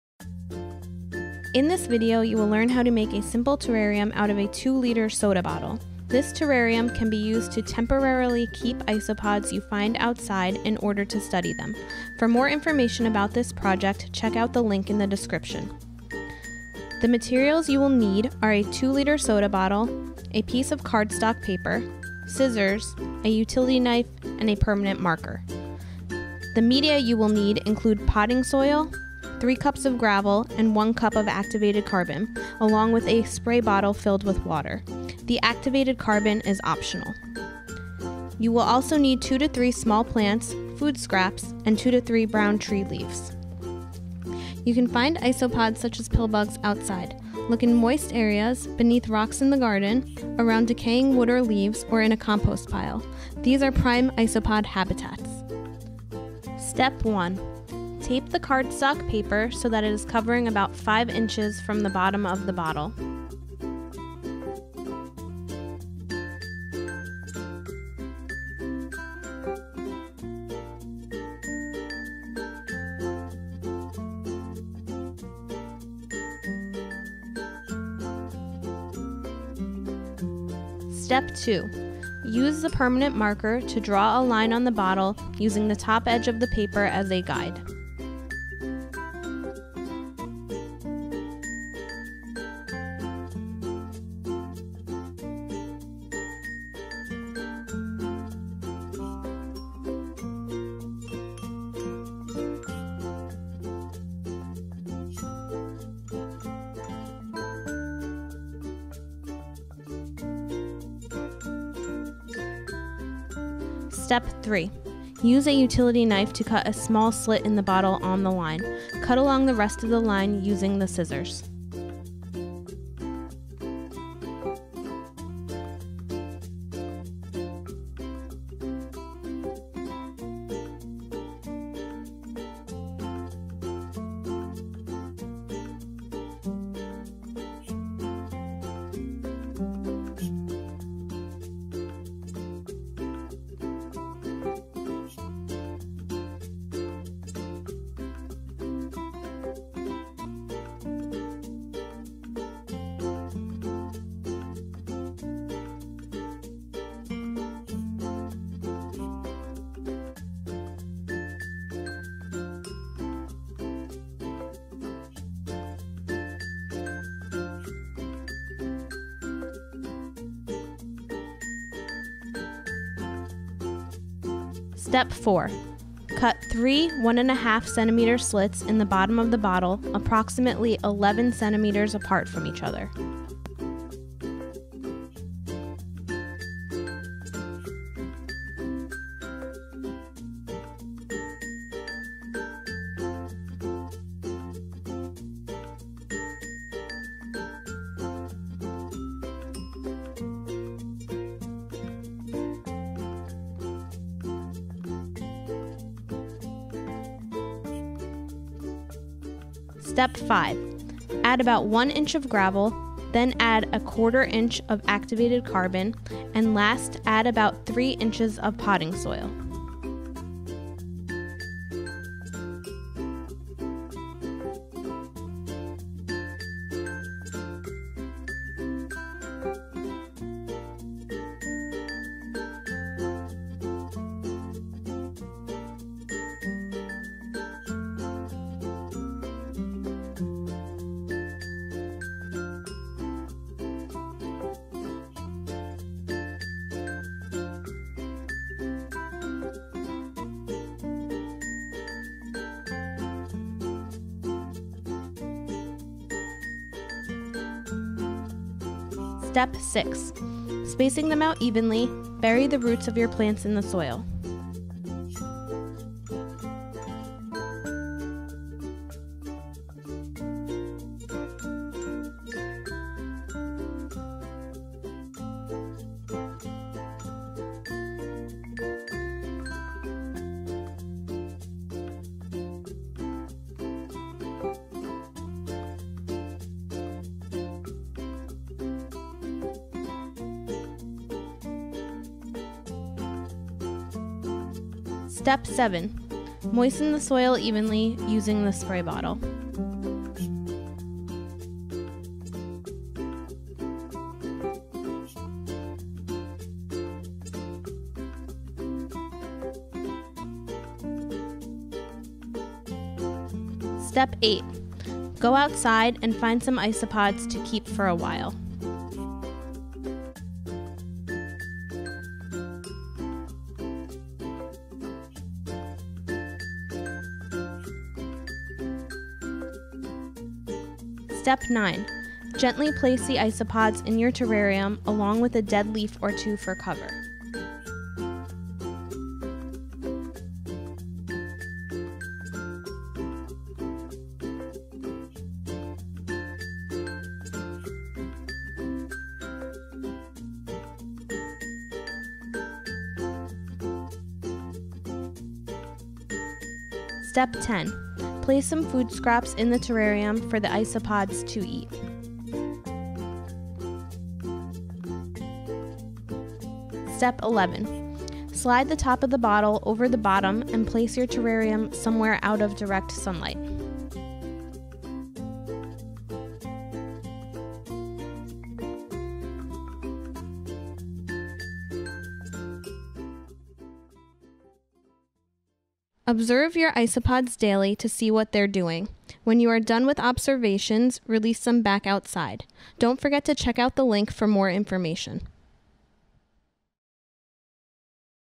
DIY Bottle Terrarium
In this video, you will learn how to create a terrarium from a plastic bottle.